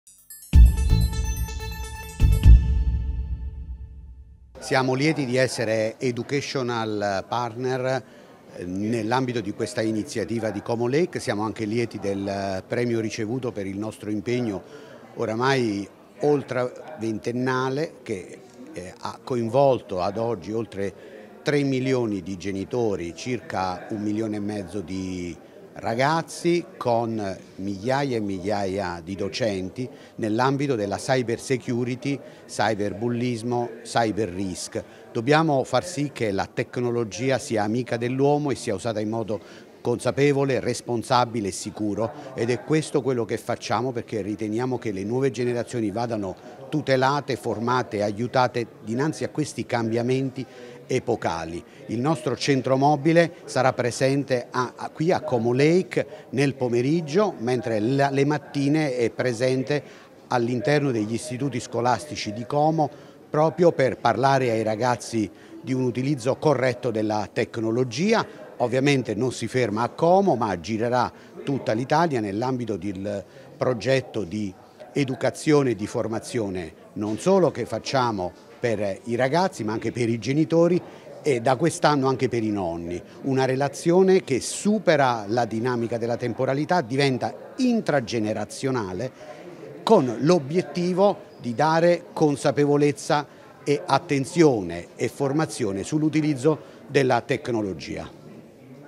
ComoLake 2024, Affinita (Moige): “Occasione per parlare ai ragazzi dell’utilizzo corretto della
“Siamo lieti di essere Education partner nell'ambito di questa iniziativa e siamo anche lieti del premio ricevuto per il nostro impegno ormai ultraventennale, che ha coinvolto ad oggi oltre 3 milioni di genitori, circa 1,5 milioni di ragazzi e migliaia di docenti, nell'ambito della cyber security, del cyber bullismo e del cyber risk. Dobbiamo far sì che la tecnologia sia amica dell'uomo e sia usata in modo consapevole e responsabile e sicuro. Ed è questo quello che facciamo, perché riteniamo che le nuove generazioni vadano tutelate, formate e aiutate”. Così Antonio Affinita, direttore generale Moige - Movimento italiano genitori, a margine della seconda giornata di ‘LakeComo 2024’, la conferenza internazionale nata per contribuire al confronto sulle politiche digitali di oggi e di domani e alla quale prendono parte istituzioni, imprese,  università e centri di ricerca.